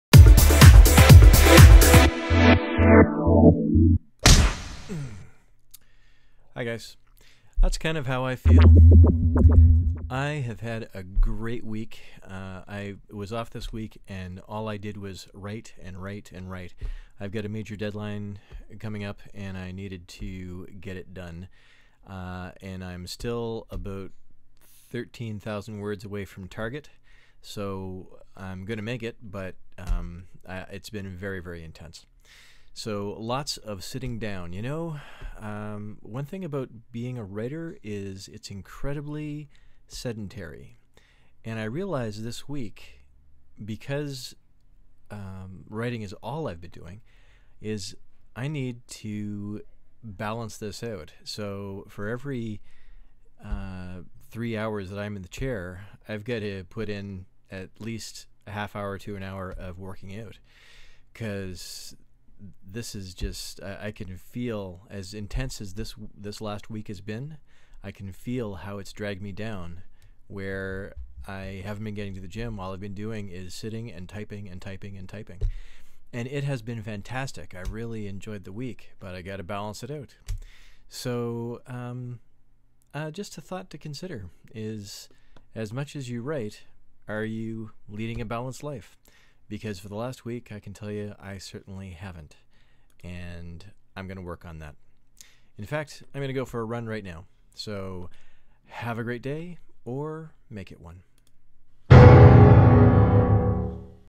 Work out to balance the writer's life
Sedentary living means a big word count, but we've got to balance it out.